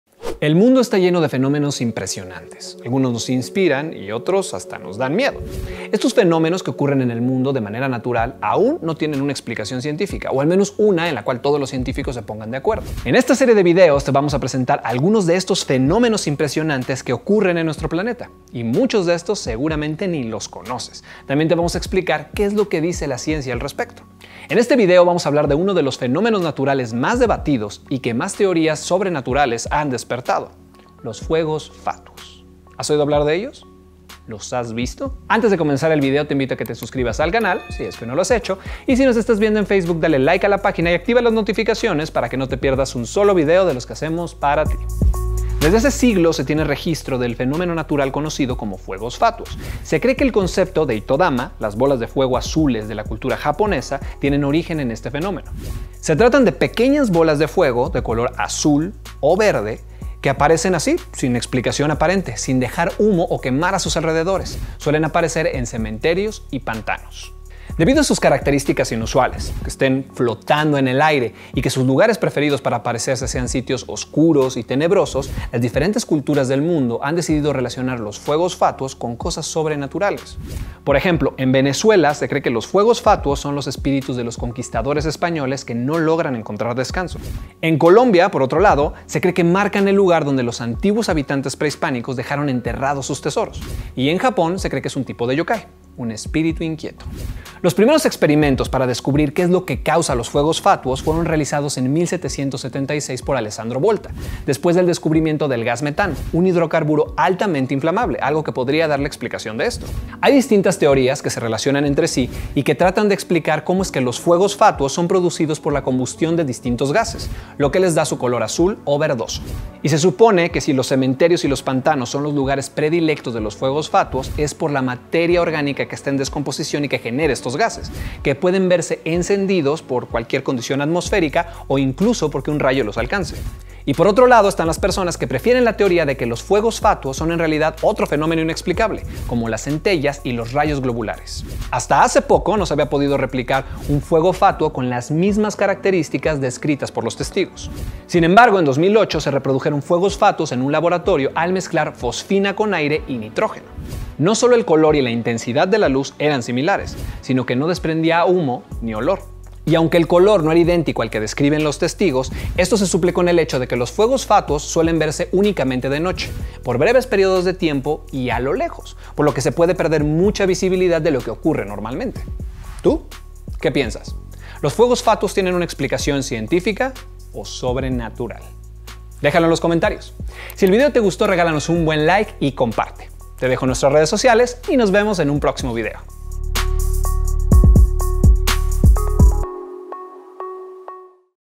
Fuegos fatuos, el FENOMENO NATURAL más PERTURBADOR | Viranimals
Hay fenómenos naturales que son difíciles de explicar pero los fuegos fatuos o también conocidos como bolas de fuego, son uno de los más extraños, inexplicables y asombrosos del planeta.